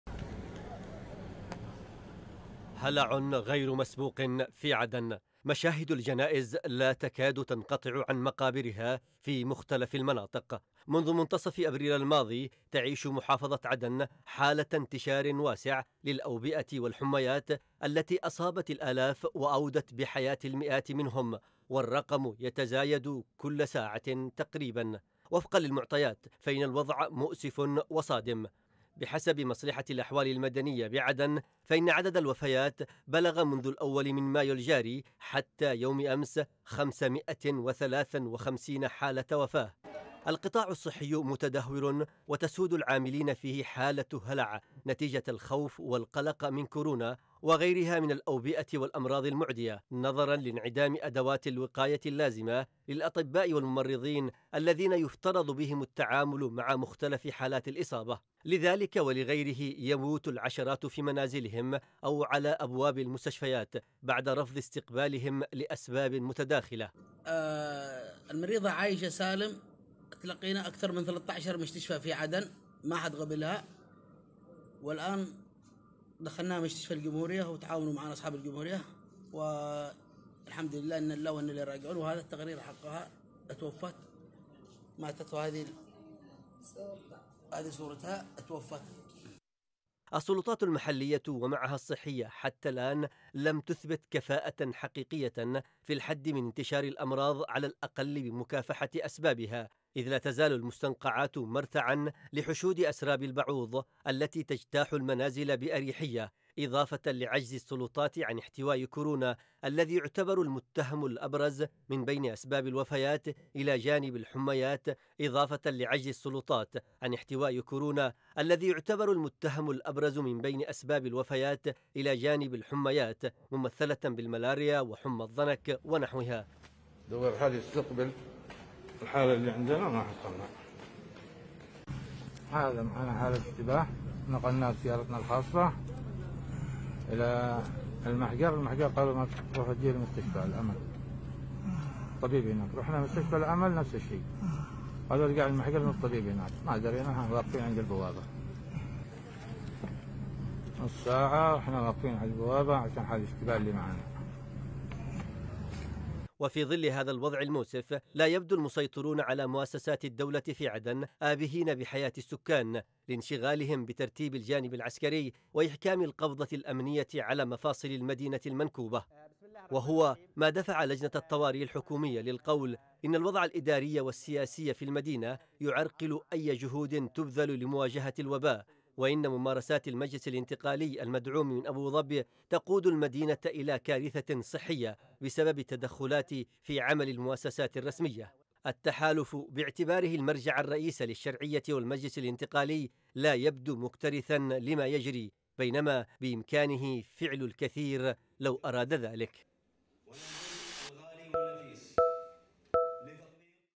الحميات وكورونا يحصدون أرواح المئات في عدن | تقرير: وديع عطا
من يوقف عداد الموت في عدن؟
ومن المسؤول عن تفشي الأوبئة والأمراض في العاصمة المؤقتة؟
وكيف يمكن التصدي لموجة الوباء الذي شمل كل مناطق عدن تقريباً؟  ..